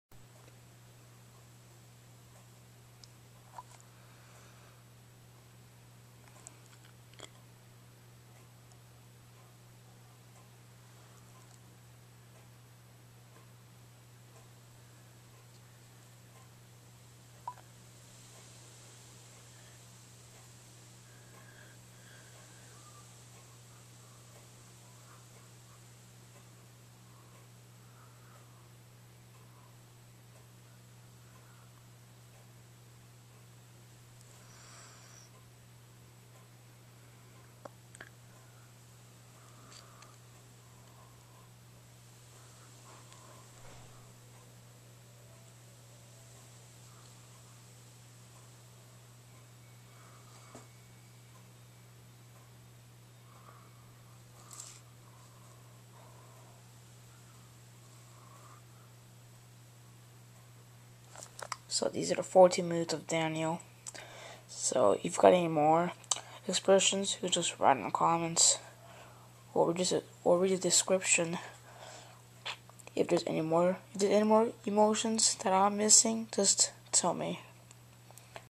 14 moods of Daniel. Got more?
Emotions of Daniel. dang, he's so emotional. If I'm missing some emotions, just write a comment of it. All anime emotes are pretty essential, anyways. :P